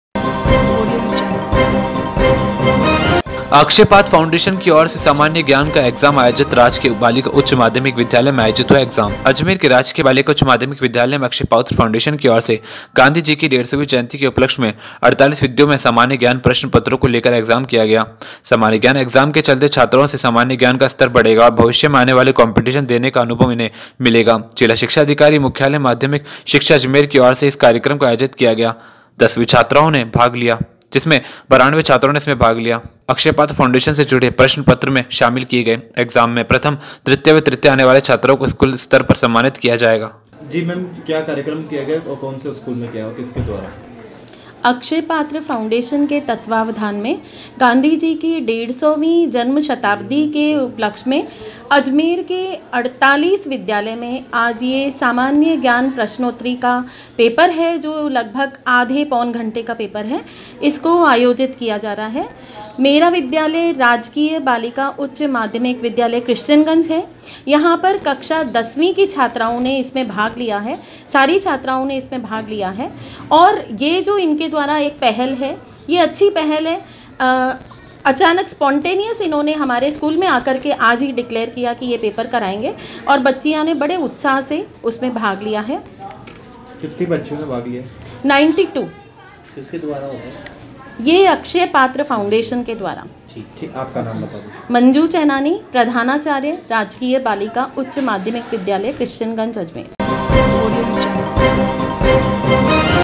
HORIZON HIND - अक्षय पात्र फाउंडेशन की और से सामान्य ज्ञान का एग्जाम आयोजित
अक्षय पात्र फाउंडेशन की और से सामान्य ज्ञान का एग्जाम आयोजित

 राजकीय बालिका उच्च माध्यमिक विद्यालय में आयोजित हुआ एग्जाम